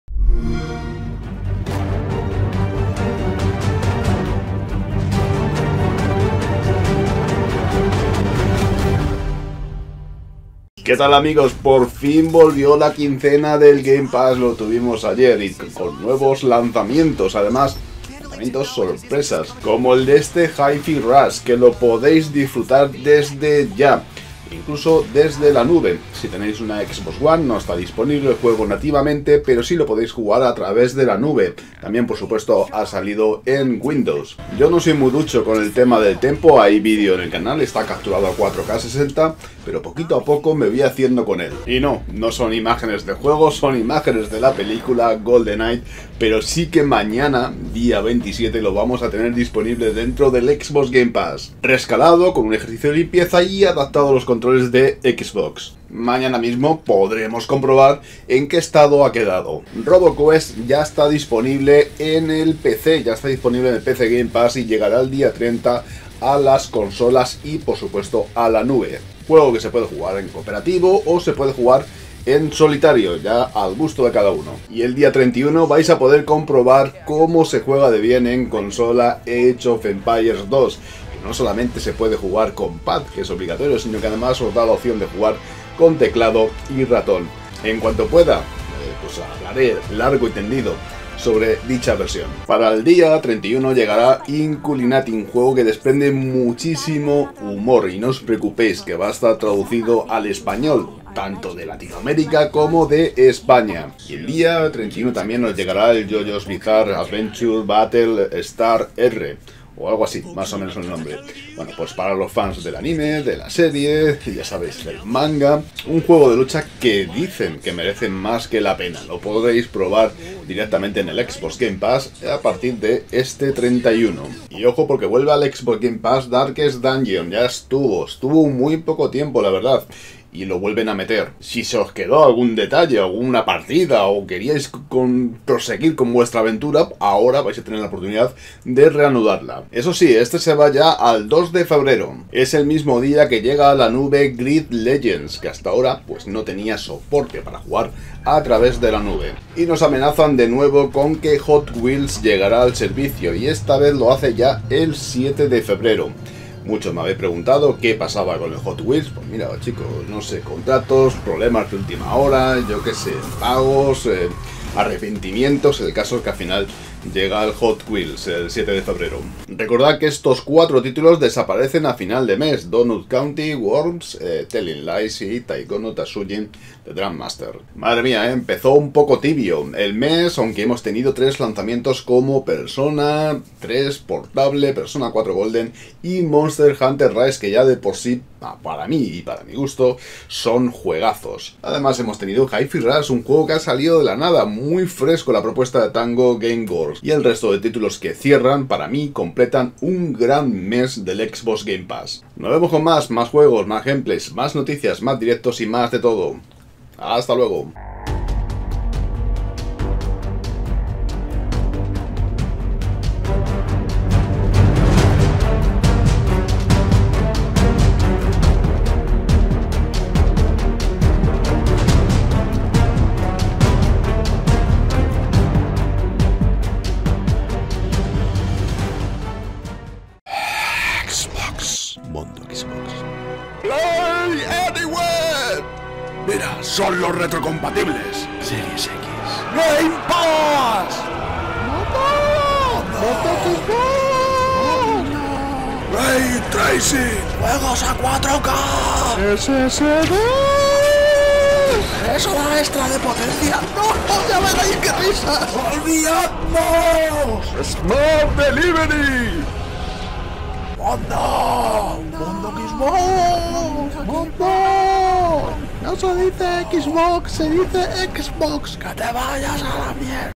Novedades en Xbox Game Pass para Enero 2023, segunda quincena xCloud, PC y Xbox.
📲 Descarga la APP de Eneba y vende juegos, consolas o PC's
Código creador Epic Store: MONDOXBOX